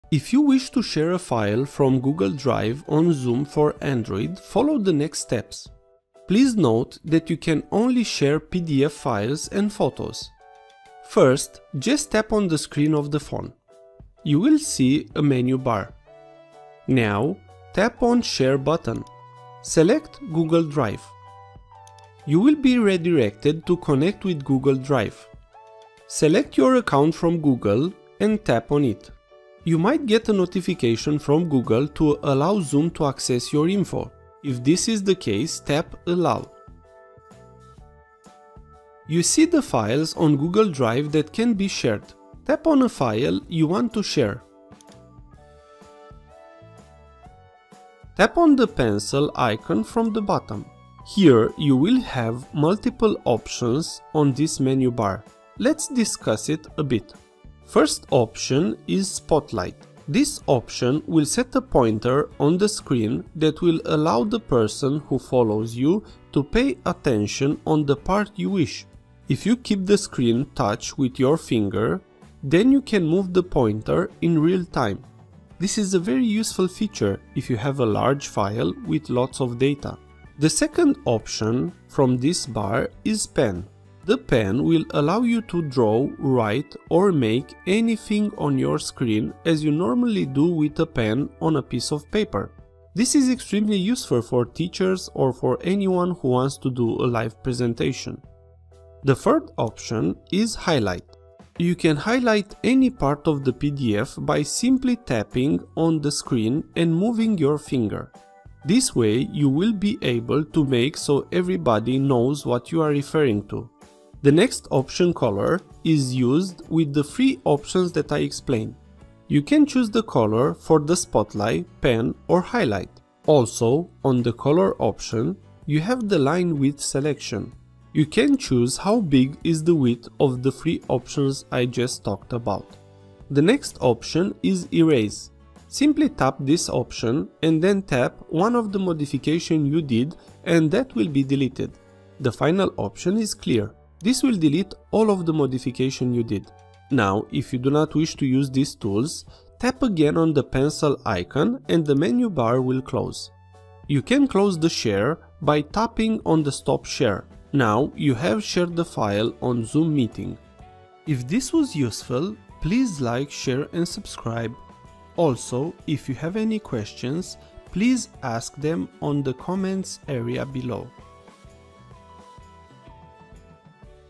How to Share a File From Google Drive on Zoom for Android
How To Share a file from Google Drive on Zoom for Android.  How to share screen a file from Google Drive on Zoom for Android

So if you wish to share a file from Google Drive on Zoom for Android, follow this step by step tutorial.